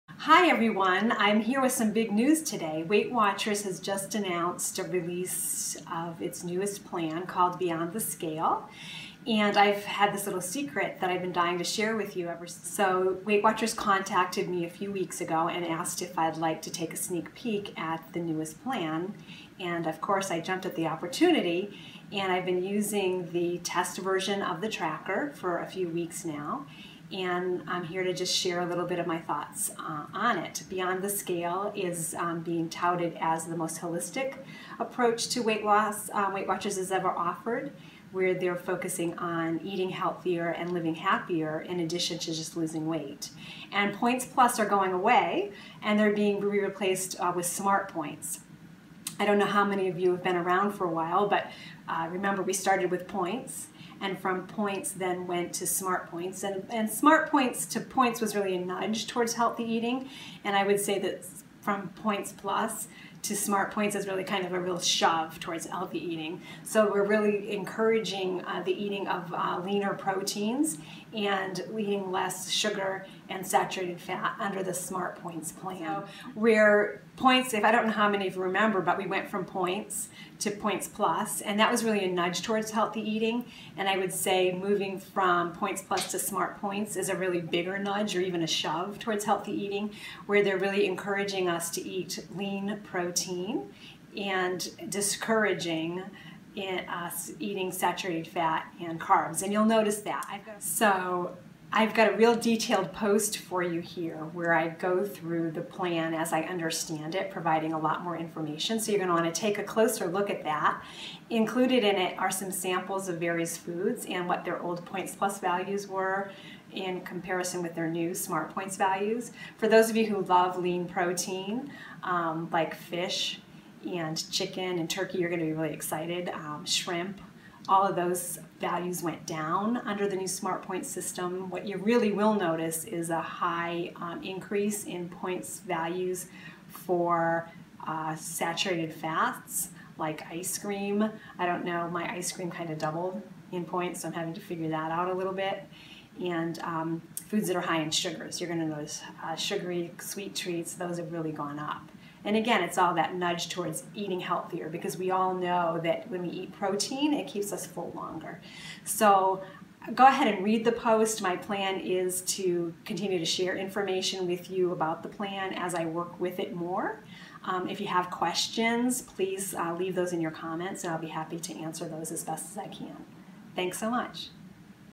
Beyond the Scale New Weight Watchers Program 2016
Martha McKinnon from Simple-Nourished-Living.com Shares Her Experience with the Brand New Weight Watchers Program: Beyond the Scale. You'll find more details on her blog